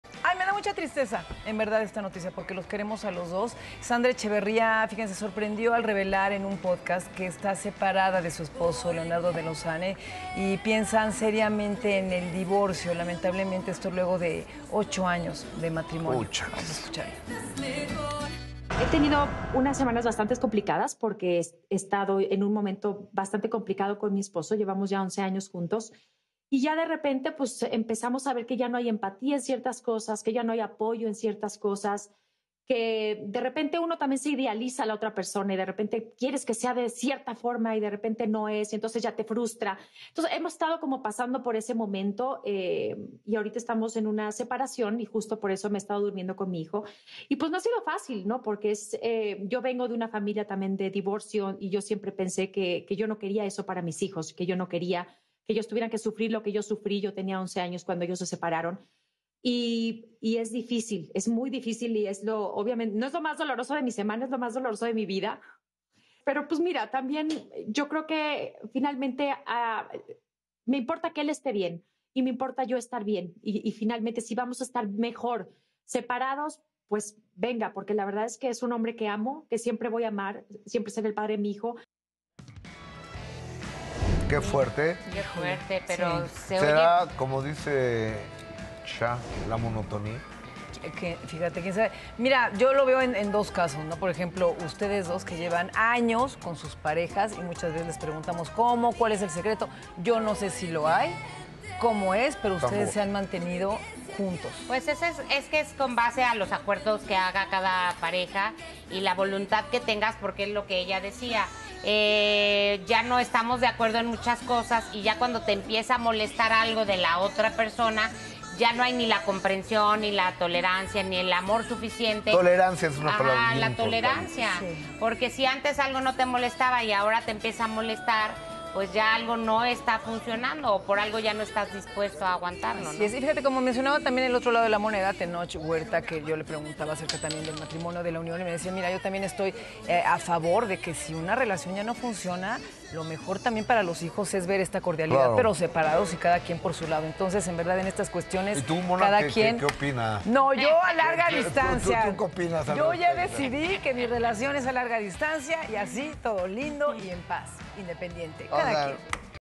'Es lo más doloroso de mi vida' : Sandra Echeverría anuncia separación con Leonardo de Lozanne
SandraEcheverría reveló que tras 8 años de matrimonio, está separada del cantante LeonardoDeLozanne. Ella revela los motivos de esta separación.